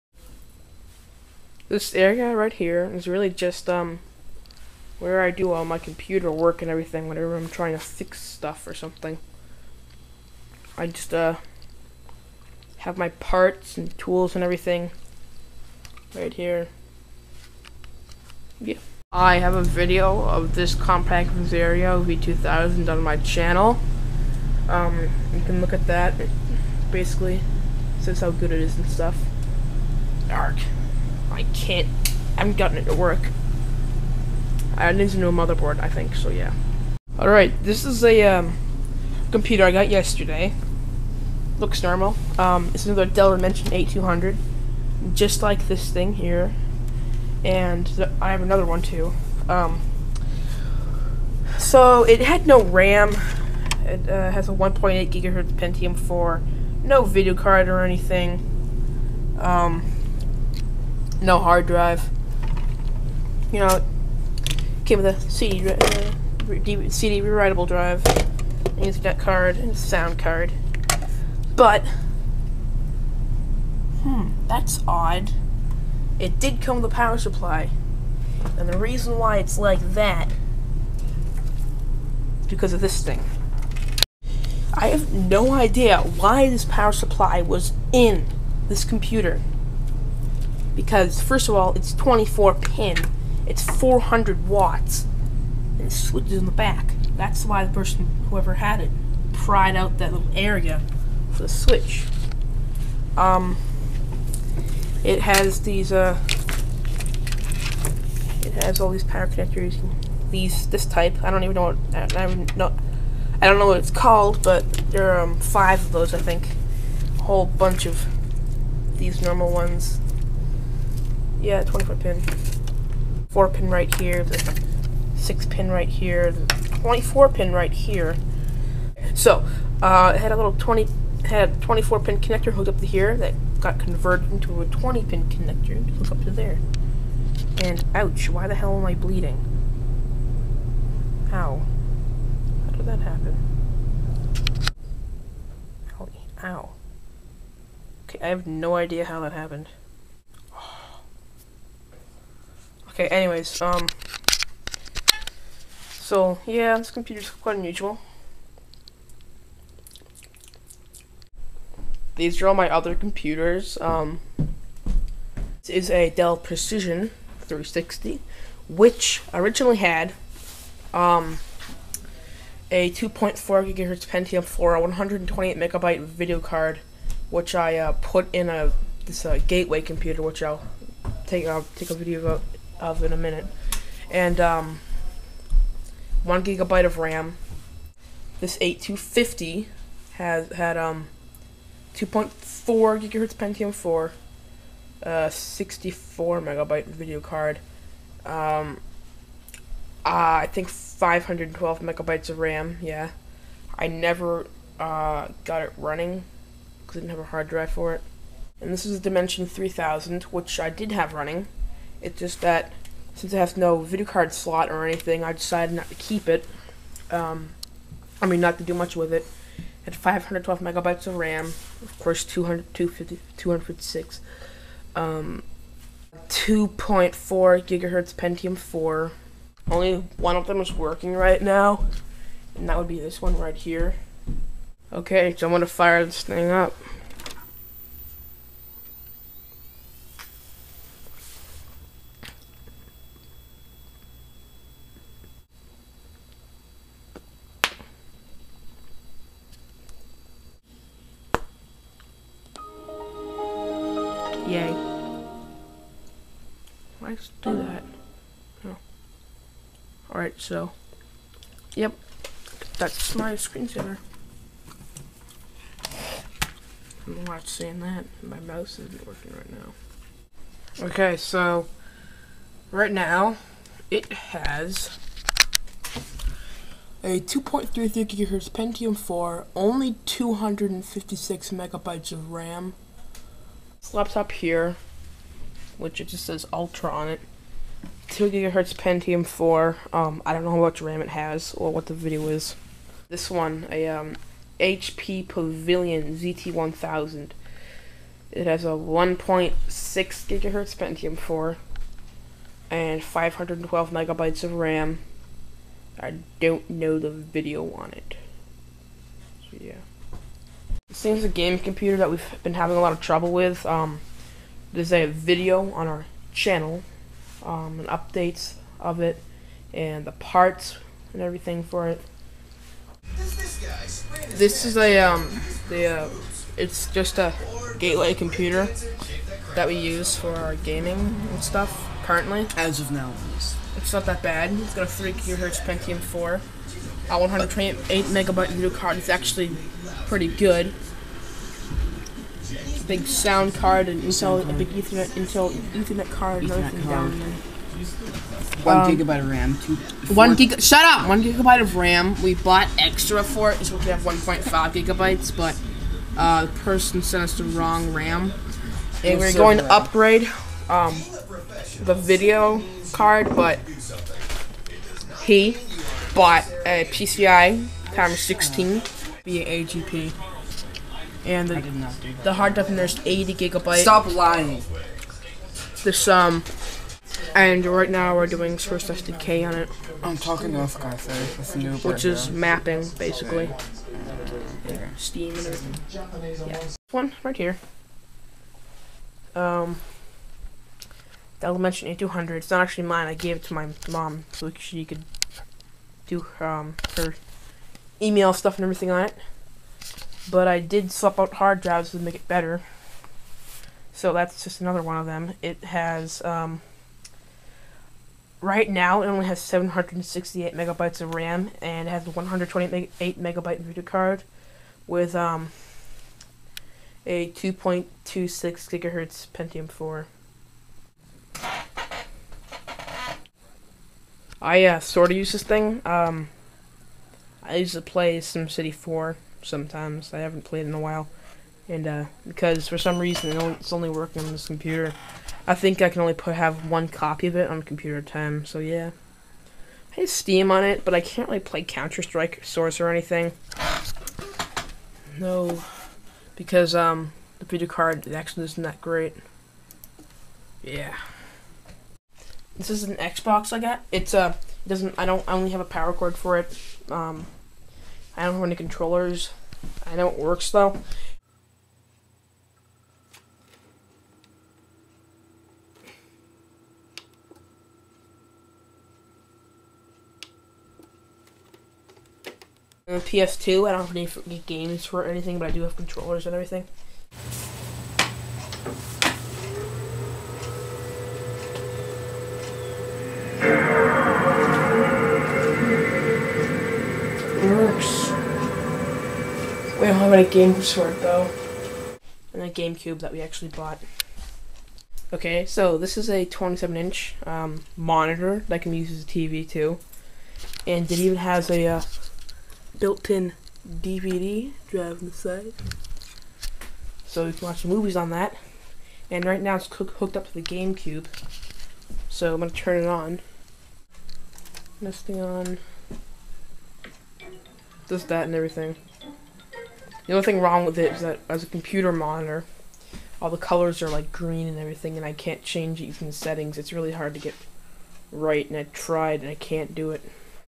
Big Computer Collection + My Gaming Computer
Computers I got trash picked:
Dell Dimension 8250
Dell Dimension 8200
Dell Dimension 8200
Dell Dimension 8200
Dell Dimension 3000
Dell Precision 360
Compaq Presario v2000
HP Pavilion zt1000
Xbox
PLaystation 2
Some computer that I can't find the name of (it just says "ULTRA" on it)